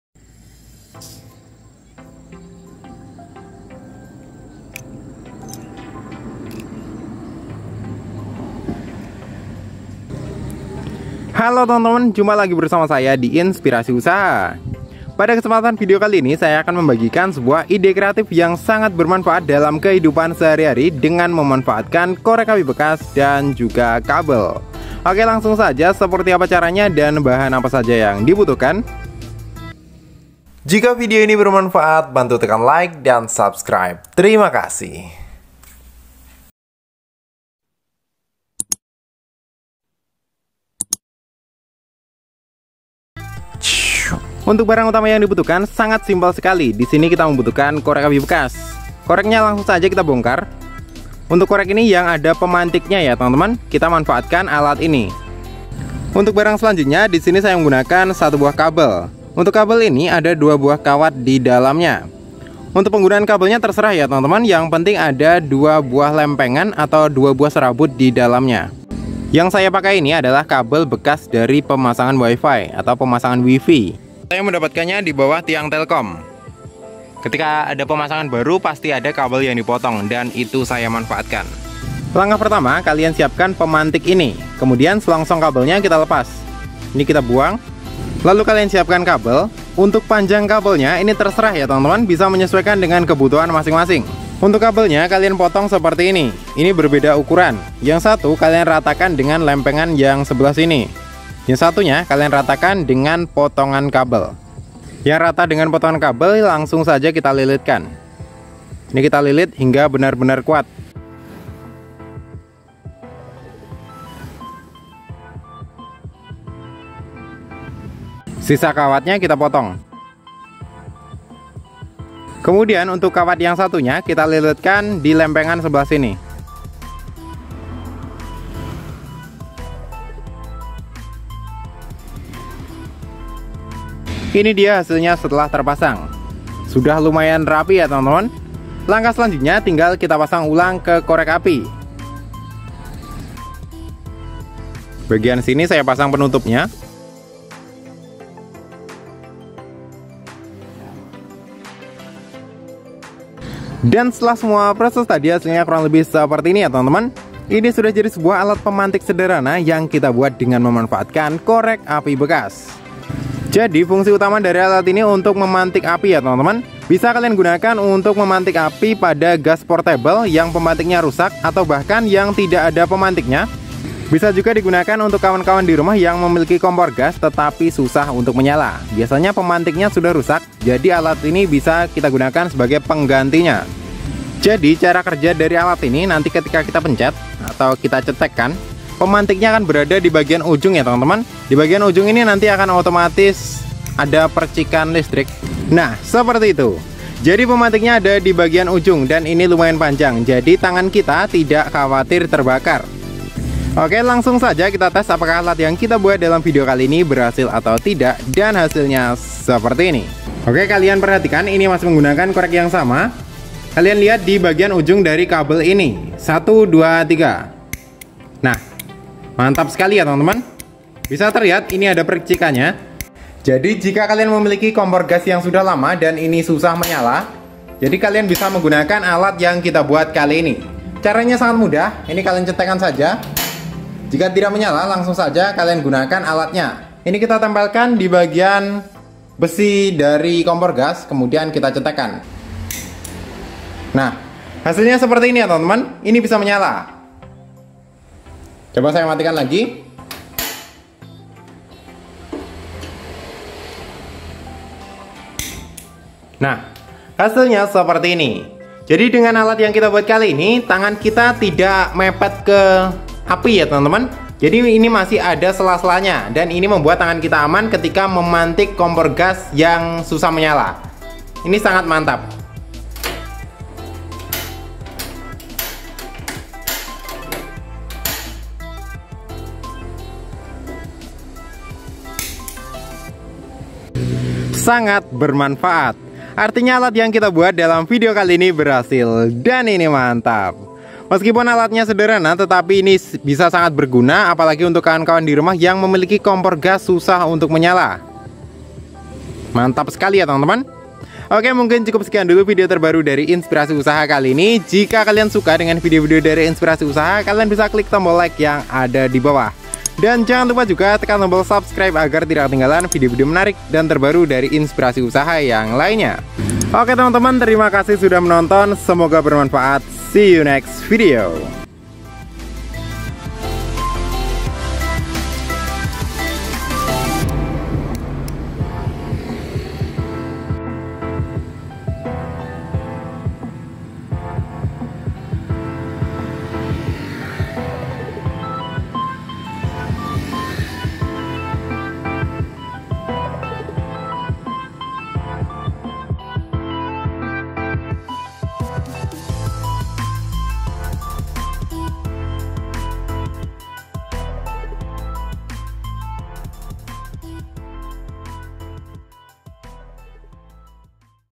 TERNYATA BANYAK YANG BELUM TAU FUNGSI LAIN DARI ALAT INI !
yang dirumahnya pakai kompor gas wajib tau trik bermanfaat seperti ini !!

bagikan vidio ini ke teman teman kalian agar lebih banyak informasi yg bisa dibagikan